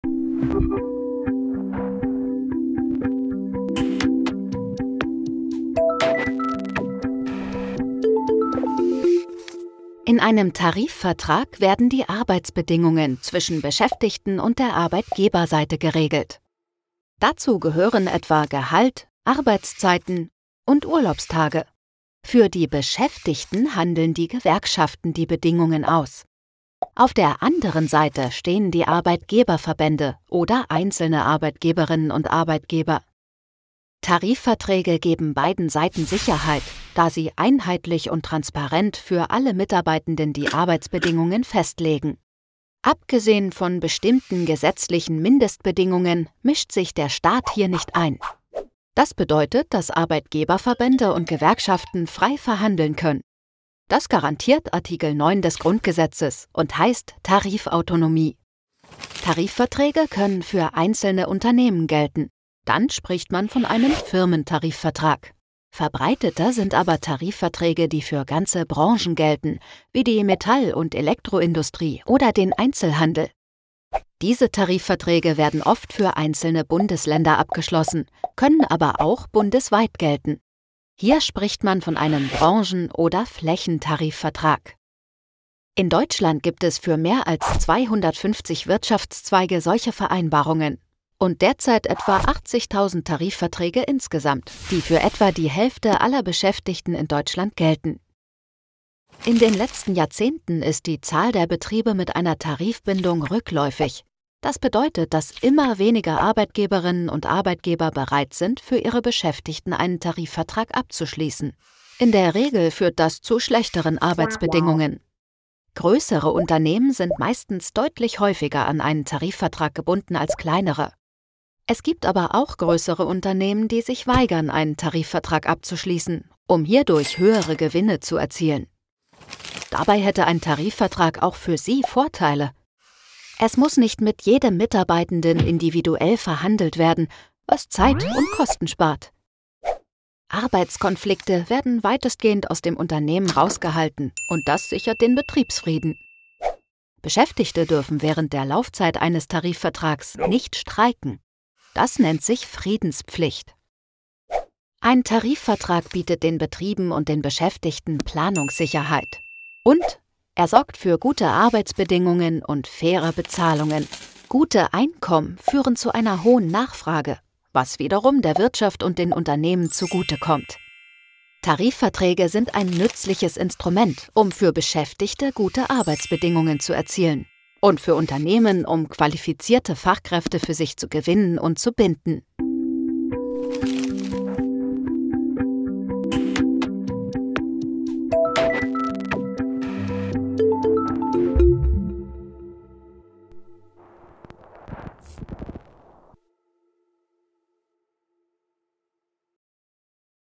Tarifvertrag – einfach erklärt
Das Video »Tarifvertrag – einfach erklärt« von BöcklerSchule erklärt, was Tarifverträge regeln, für wen sie gelten und wer sie aushandelt – außerdem zentrale Begriffe wie Tarifautonomie und Friedenspflicht.

Ein Film der Hans-Böckler-Stiftung. Produziert von explainity®

Böckler Schule ist das Lehrerportal zur sozioökonomischen Bildung der Hans-Böckler-Stiftung.